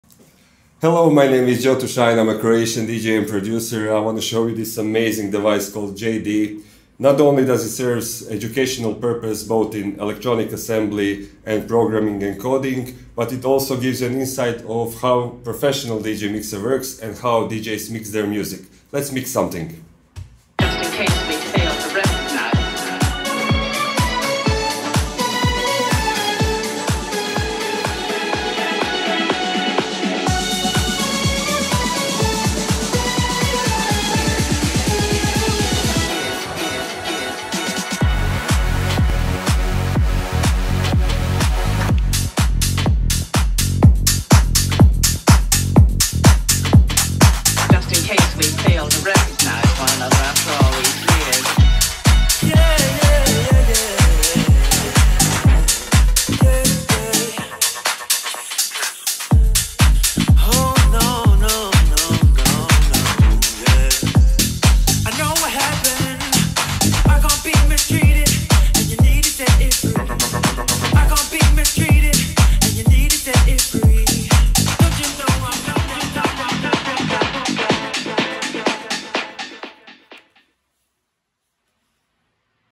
Joe2Shine jamming on Jay-D
Many thanks to Joe2Shine for showcasing his skill on our DIY mixtable.